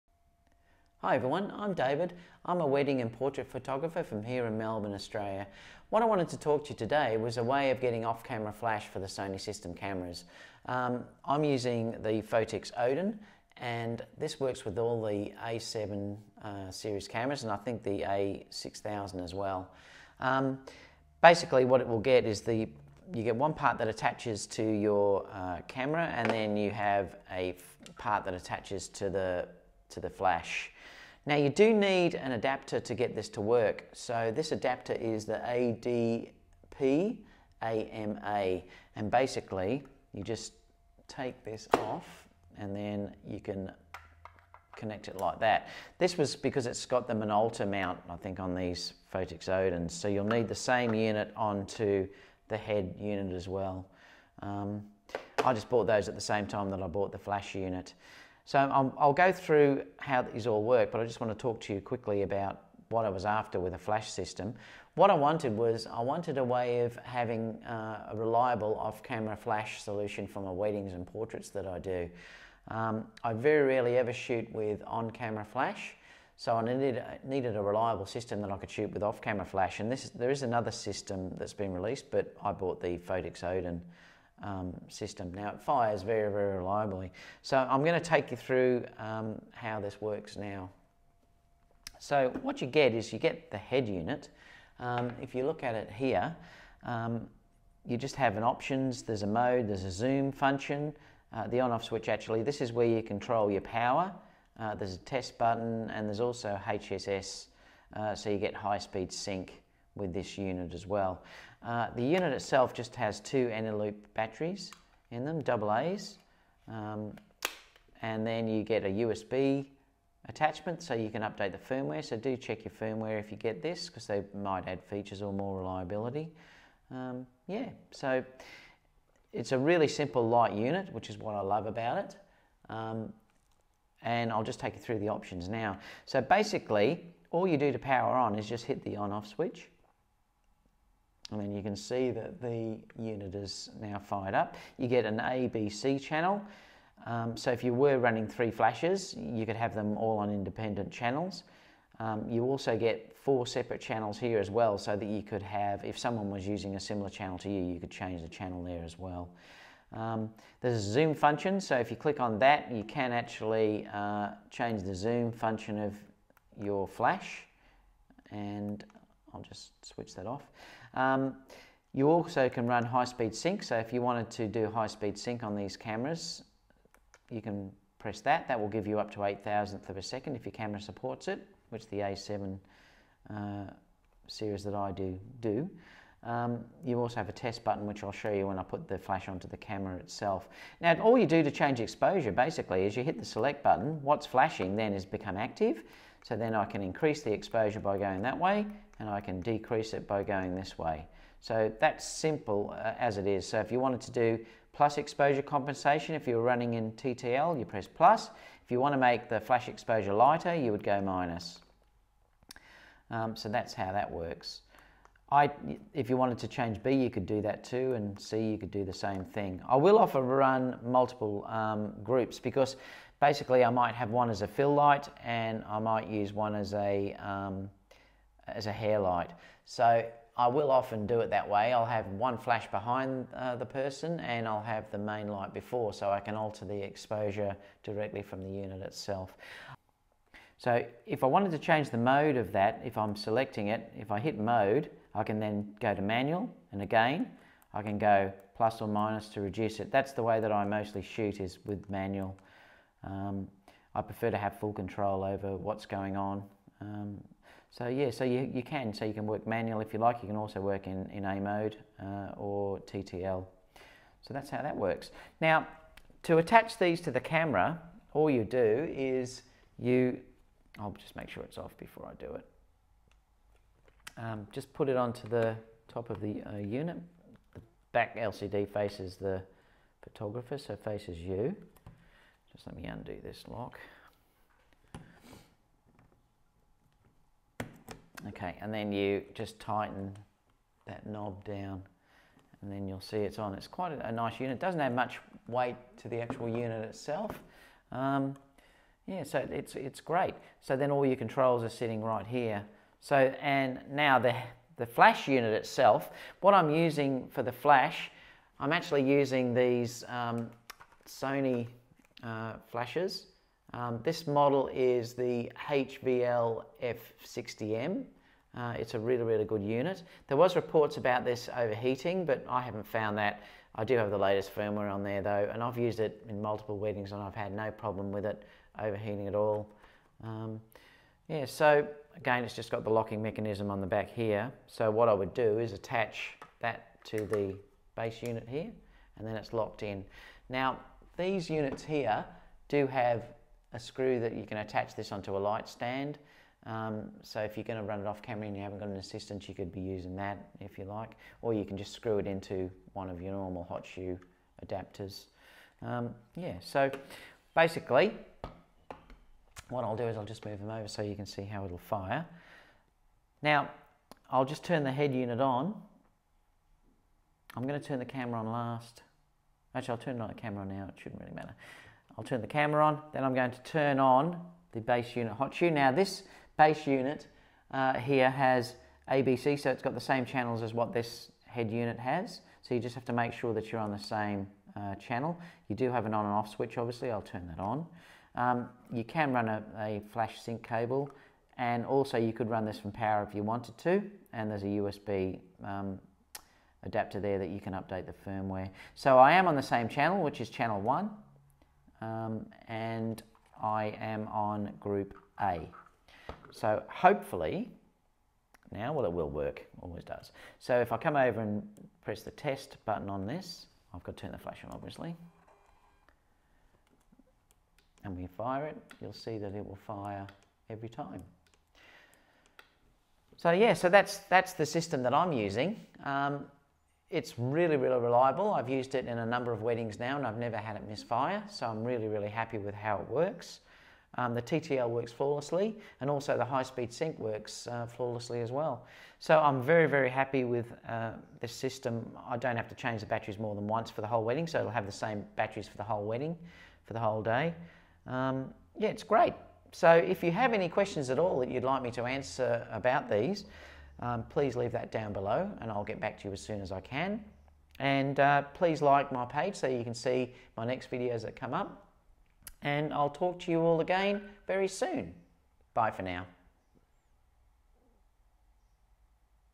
Off camera flash for Sony E mount using Photix Odin wireless triggers
Off camera flash for Sony E mount cameras using the Photix Odin wireless trigger system
The hotshoe adaptor